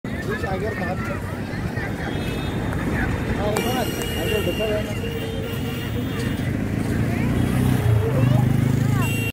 HD & 4K Royalty free stock video footage
Here is a royalty free stock video footage of "Puri Sea Beach "  from X-fActor: Royalty free stock video footages

You can use this royalty free stock video footage of "Puri Sea Beach" for personal or commercial purposes. For YouTube channel or any projects.